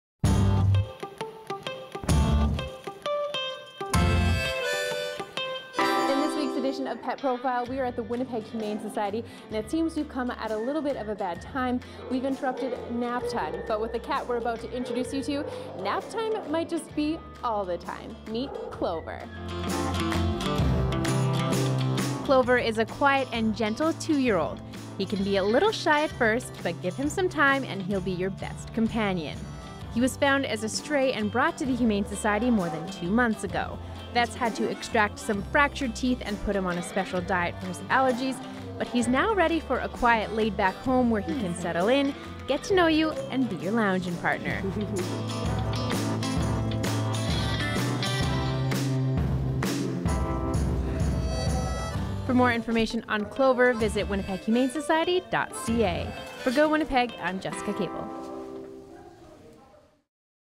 Pet Profile: Clover
A quiet, gentle two-year old kitty, Clover is looking for his forever home at the Winnipeg Humane Society - even if he might be taking a few naps along the way.

To adopt Clover, or another of the WHS's adorable adoptables,